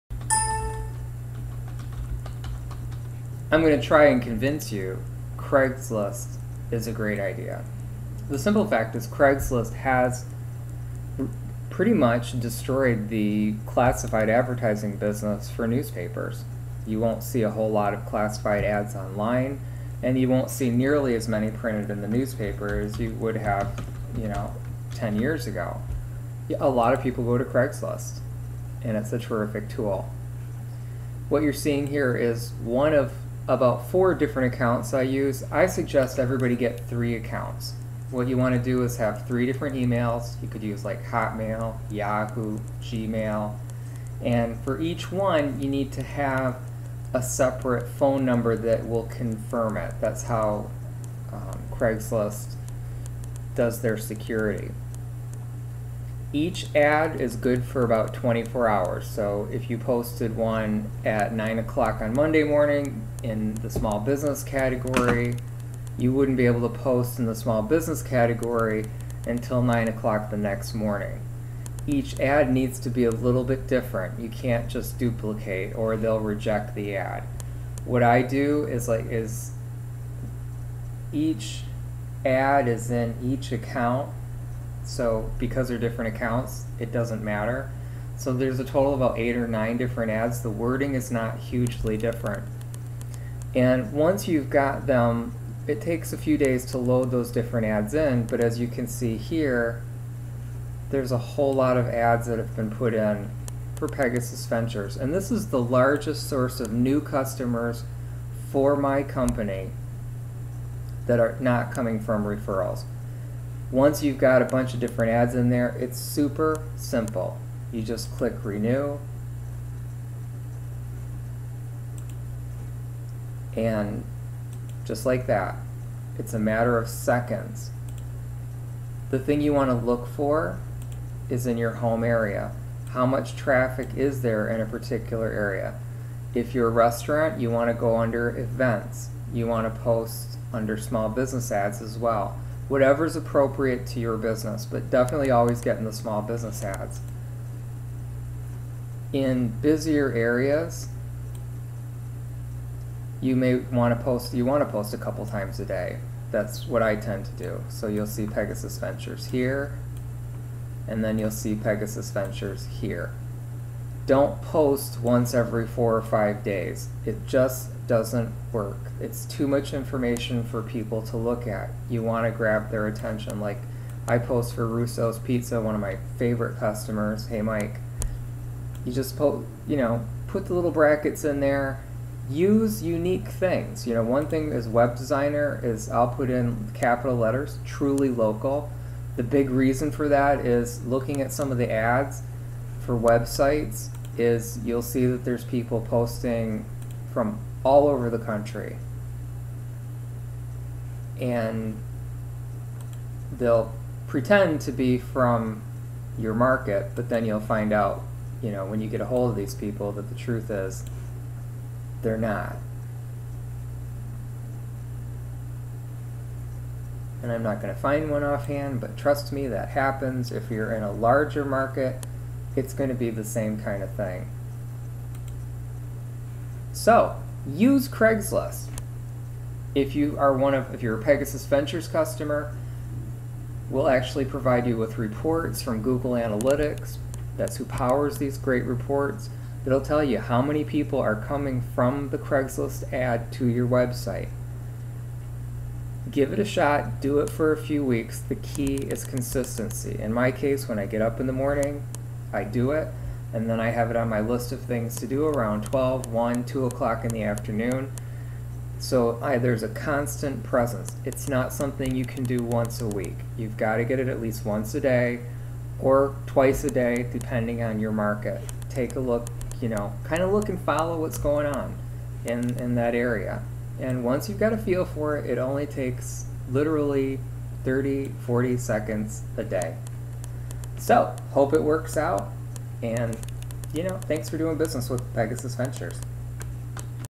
Using Craigslist to Make Money
Pegasus Ventures has a great idea for making money, it's something  that's been around a while...Craigslist. It is one of the best sources of business here at Pegasus Ventures and done well it can work for your business, too!